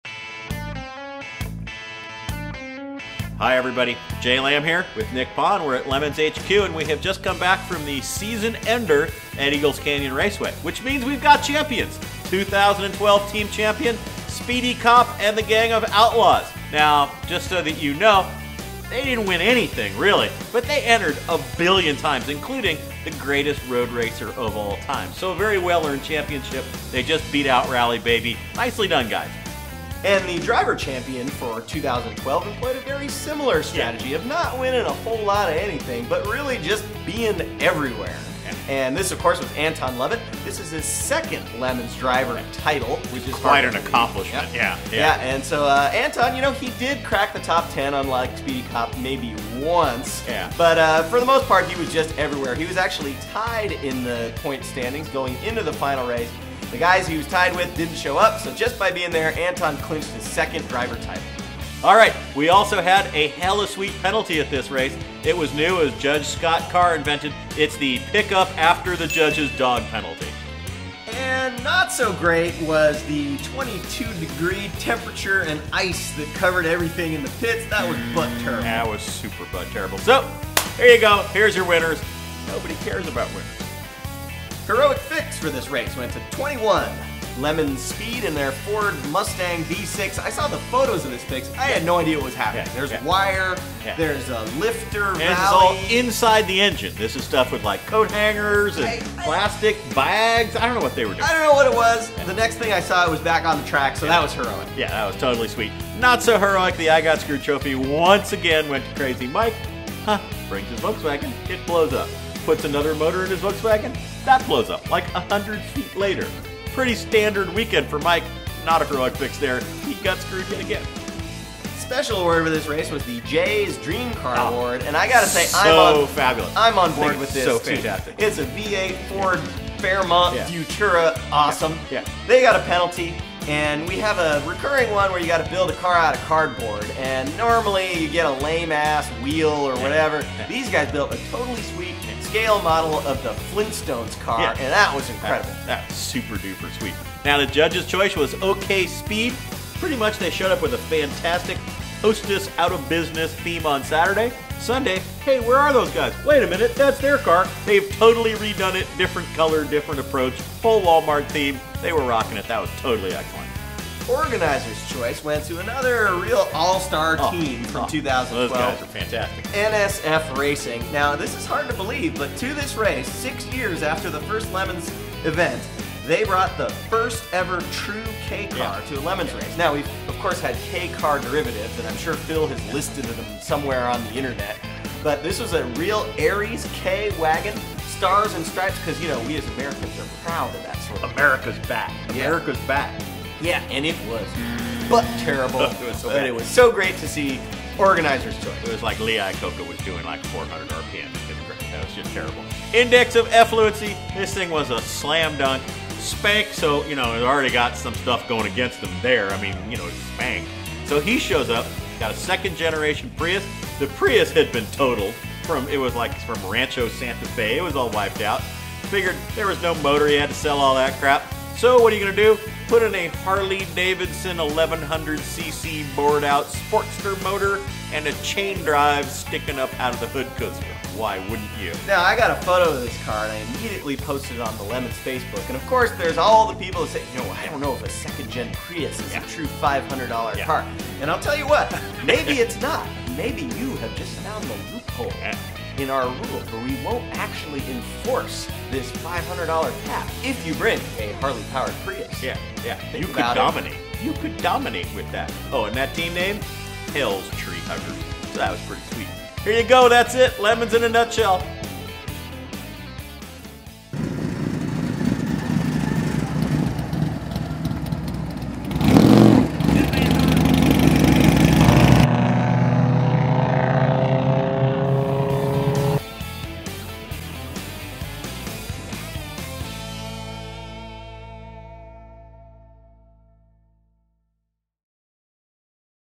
LeMons Recap-ECR #2 (Season Finale) 2012
Highlights from the Heaps in the Heart of Texas at Eagles Canyon Raceway in Decatur TX, December 2012